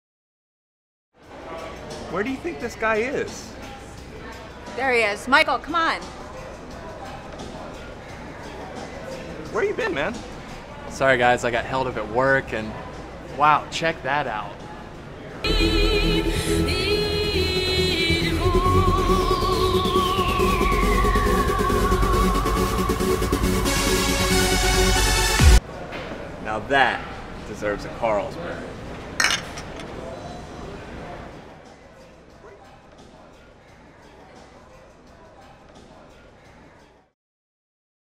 Pandoodle Viral Marketing Demonstration with Carlsberg
Pandoodle's proprietary software enables video producers and distributors to customize digital media so quickly, cheaply, and well that it presents a new form of online advertising and new opportunities for viral marketing. Advertisers and marketers can enrich online video with hyper-targeted images and video clips that seem to be part of the original film yet address specific regions or individual viewers. For example, if in a movie, two men are drinking beer in a bar, the logos on their beer bottles can adapt automatically to the location of where the movie is being shown. In Columbia, they might display Carolina Blonde, in Boston, Sam Adams, in India, Kingfisher, in Japan, Kirin, and so forth. If a TV show is being streamed to a 40-year old woman in New York, she might see a poster in the background advertising the latest Broadway show, whereas a 21-year old in Indianapolis might see one advertising the latest action flick. Think Google Adwords for brand placement within digital media.

Our patented technology is new, radical, and unique to Pandoodle, and you can see its implications for viral marketing in this YouTube video. In the clip, friends meet for a beer a local pub. In the background is a television playing a video clip; however, during filming, the television was never turned on. Rather placed on its screen was Pandoodle's AdProp, a printed pattern of trapezoids. When Pandoodle's application platform rendered the raw film, it instantly substituted the trapezoidal pattern with a soccer video that a user selected and uploaded. Instead of the AdProp, viewers see furnished images or video clips that have seamlessly adapted to their environment, conforming to lighting, shadow, position, motion, and focus. Users can have control over what images or videos are inserted into an advertisement and can then share that video with others, generating personalized viral marketing campaigns. You are welcome to try out the process for yourself on Pandoodle's demo